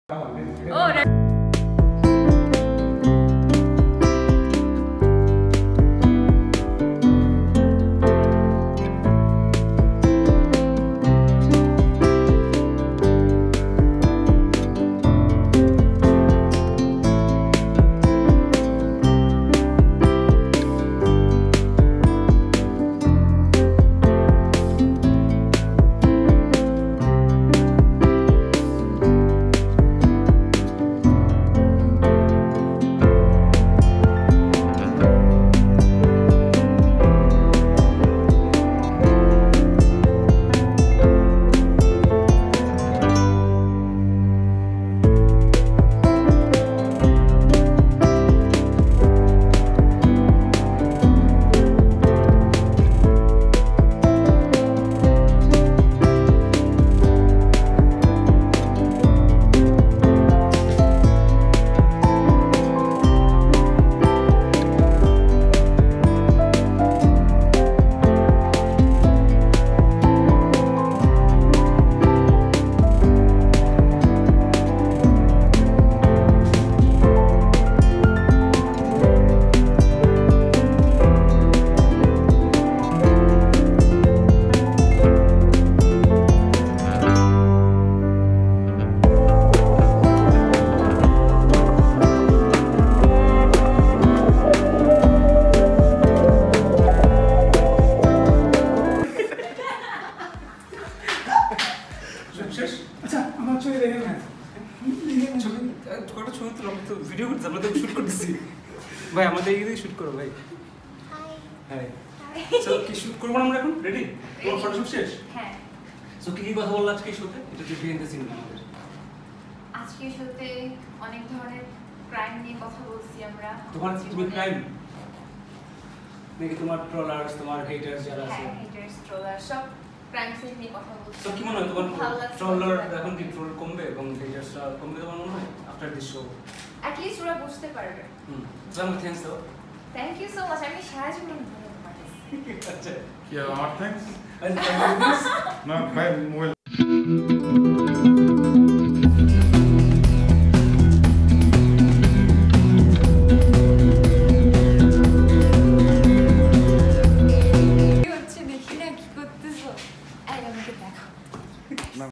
মারিয়া নুনীর ফটোশুটে কী দুষ্টুমি করলেন তানজীব সারোয়ার!| Mariya Nooni | Tanjib Sarowar | Manju Ahmed
This Is A Fun-Video Of Mariya Nooni & Tanjib Sarowar With Manju Ahmed During Their Shooting.